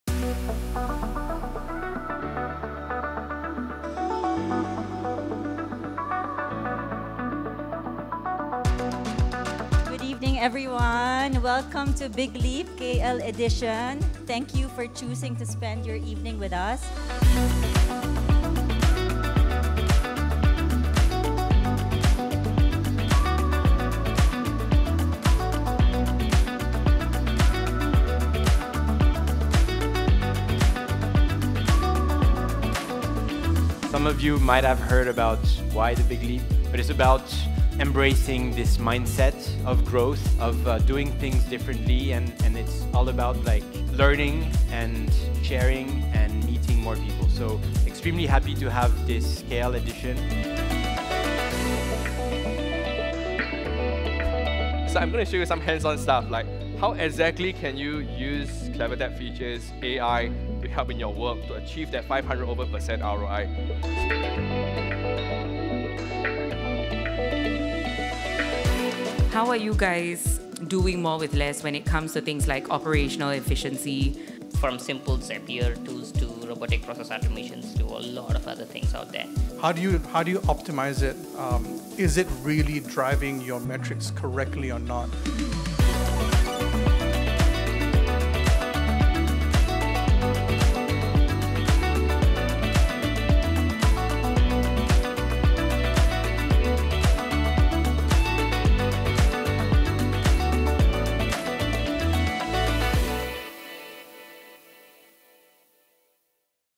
BigLeap Malaysia Highlights: Leveraging Marketing Budgets to Maximize ROI | CleverTap, AWS & Adjust
Ever wondered what it feels like to take a BIG LEAP? 🌍✨ Watch the best moments from Big Leap Malaysia and get a sneak peek of what's coming to Vietnam. Join us on August 20th, 2024, from 5pm to 8pm Vietnam Time (GMT+7) at JW Marriott Hotel, Hanoi, to discover how to leverage your marketing budget efficiently in uncertain times. Don’t miss your chance to leap into the future. 🚀🎉

Amazon Web Services (AWS) | Adjust

Mai Vo, Duke Nguyen, Marc-Antoine Hager, Bing T., Sangeeta Jash, Wei Li Saw, Annie Bo